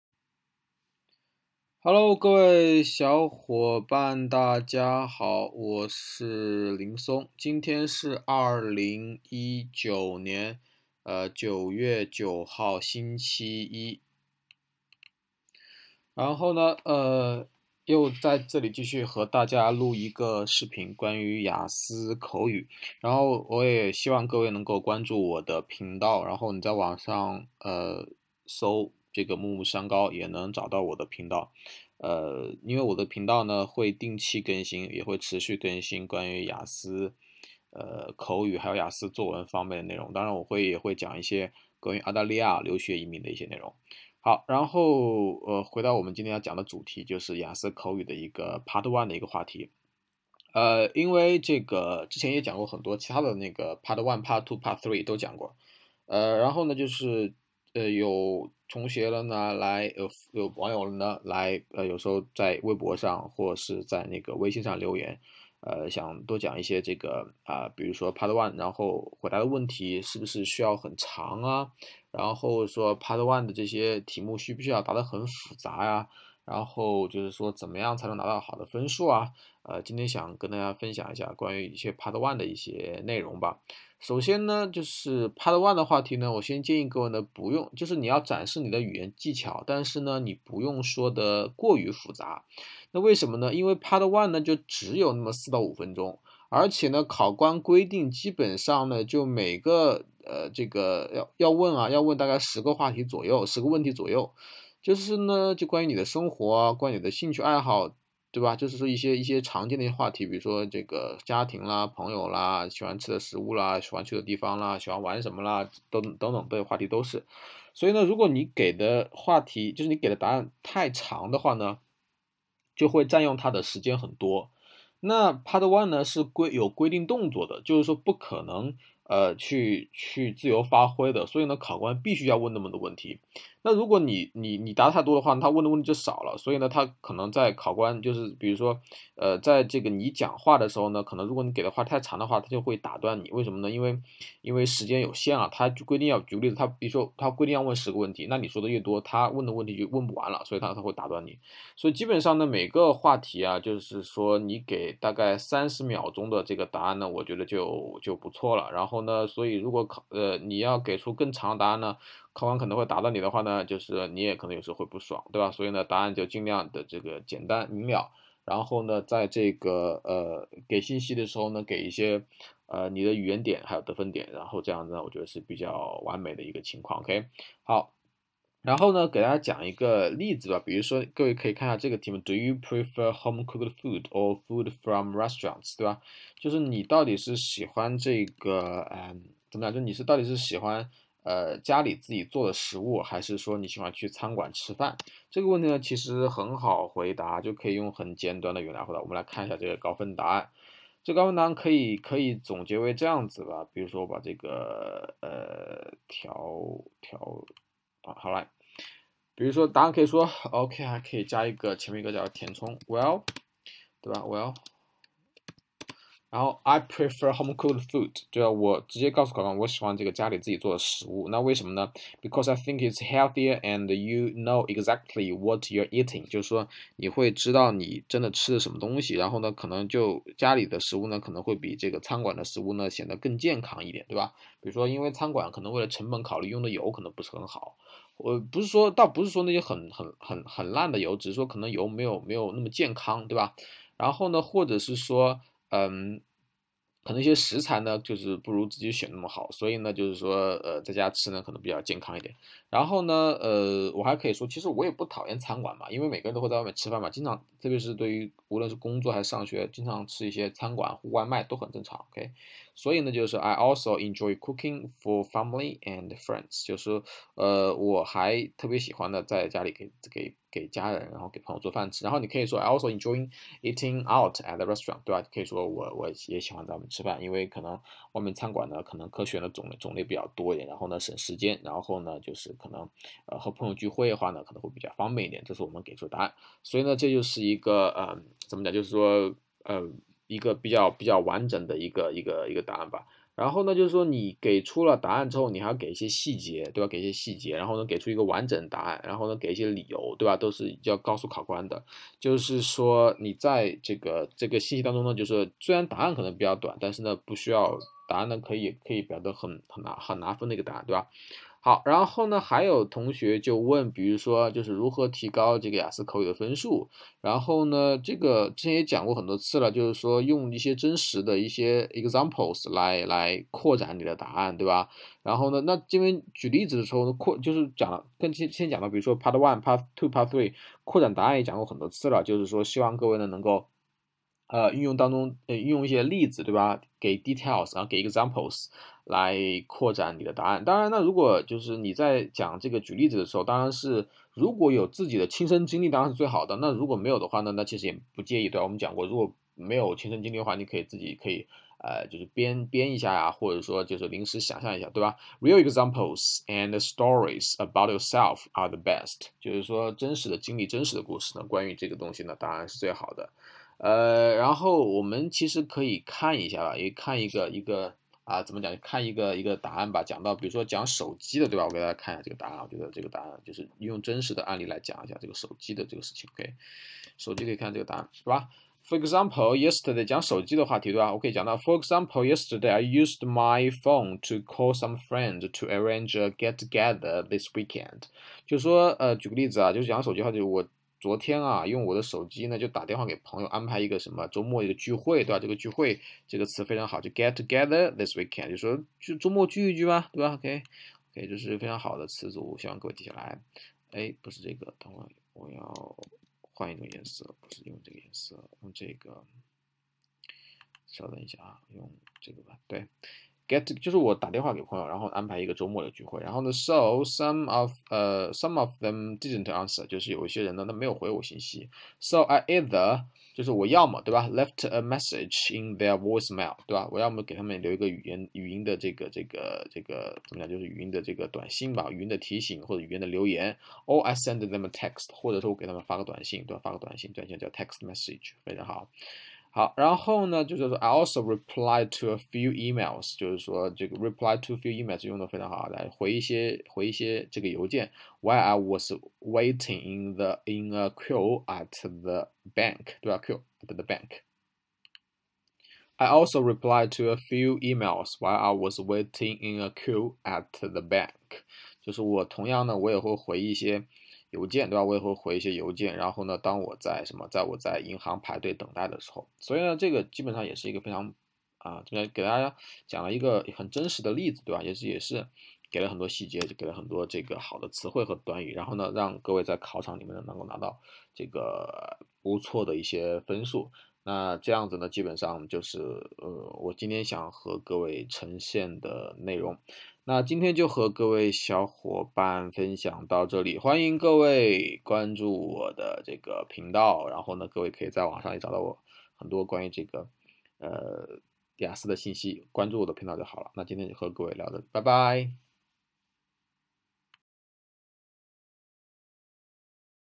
IELTS Speaking 雅思口语满分答案 0119 雅思口语第1部分答案可以简短 不用太长 第2部分用真实案例扩充答案
（本群只作为与本频道内容相关的技术交流，不闲聊）